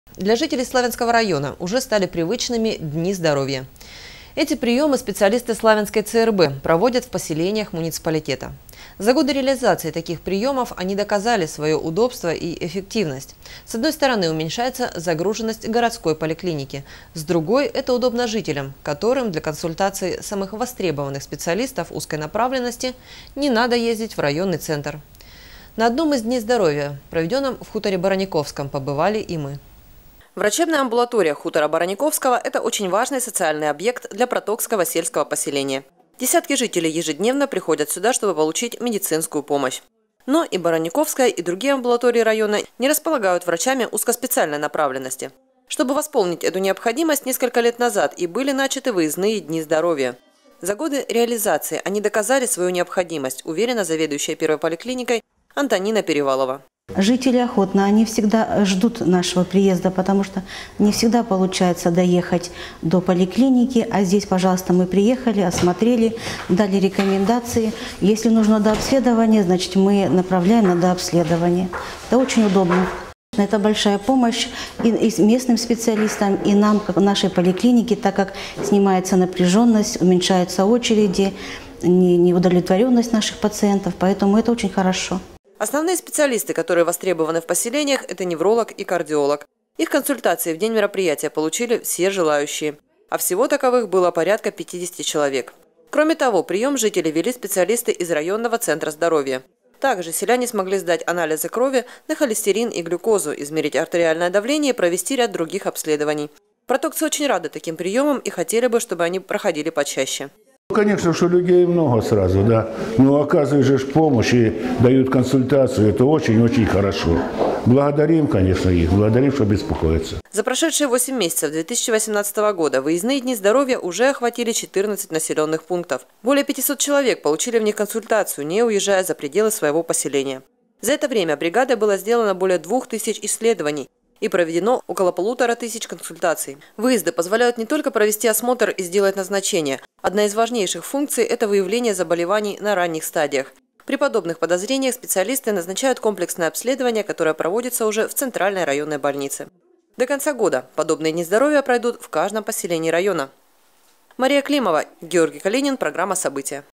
В поселениях Славянского района регулярно проходя выезды специалистов ЦРБ в рамках Дней здоровья
Для жителей Славянского района уже стали привычными Дни здоровья. Эти приемы специалисты Славянской ЦРБ проводят в поселениях муниципалитета. За годы реализации таких приемов, они доказали свое удобство и эффективность. С одной стороны уменьшается загруженность городской поликлиники, с другой - это удобно жителям, которым для консультации самых востребованных специалистов узкой направленности, не надо ездить в районный центр. На дне здоровья, проведенном в хуторе Бараниковском, побывали и мы